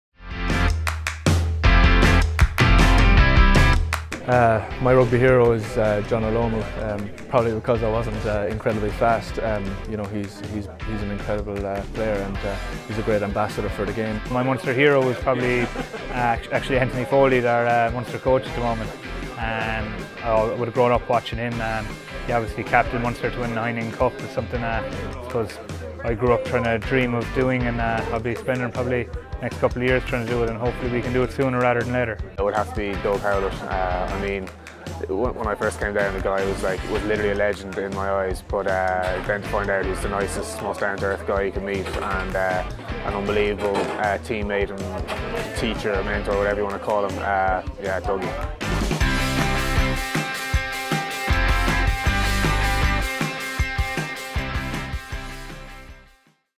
Munster Rugby Heroes
Felix Jones, Dave Foley and Dave Kilcoyne of Munster talk rugby hero’s and inspirational figures in the development of their careers.